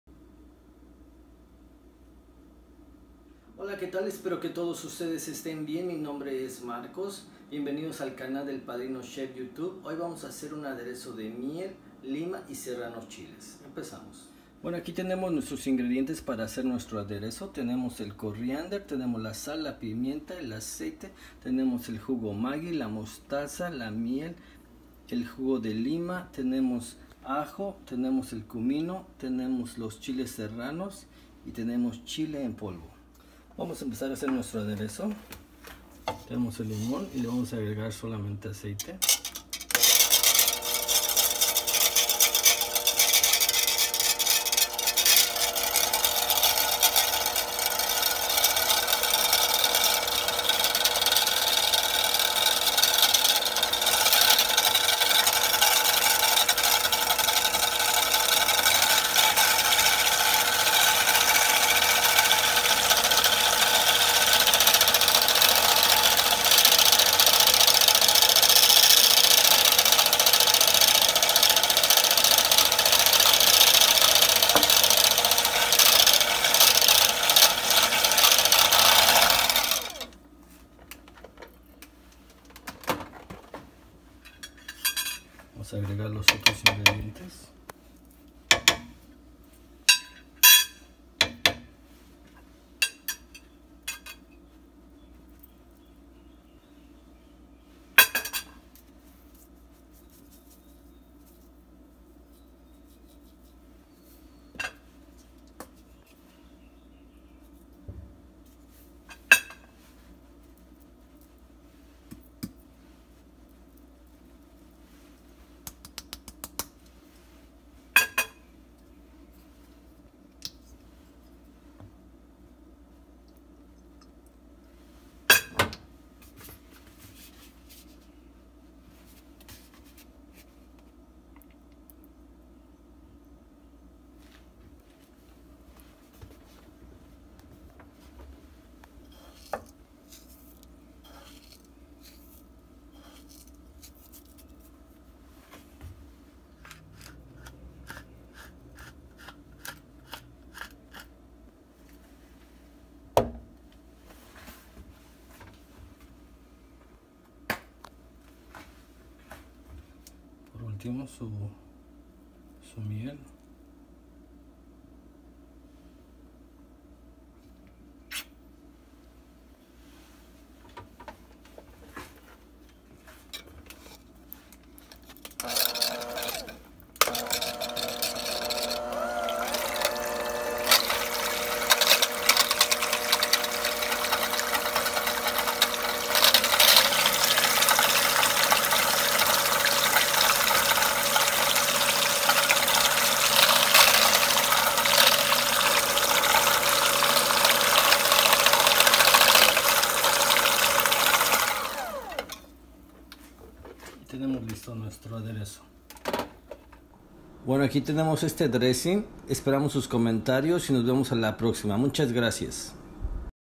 Honey Lime Serrano Chili Dressing - El Padrino Chef
El Padrino Chef
Amazing news! We are so excited to share our recipes.
From our kitchen to yours.

Yields 1 Serving
Honey Lime Serrano Chili Dressing
   • 6 oz lime juice
   • 2 oz grape seed oil
   • 2 tsp kosher salt
   • 3/4 tsp coriander ground
   • 1/4 tsp cumin ground
   • 1/2 tsp black pepper fresh
   • 1/2 tsp garlic powder
   • 1/2 tsp chili powder
   • 2 oz honey
   • 16 ea walnut halves
   • 1/2 tsp mustard
   • 1/2 tsp maggi juice
   • 1 ea serrano chili finely diced

We hope you enjoy this video and share it to keep our Mexican cooking tradition.
Gracias,
El Padrino Chef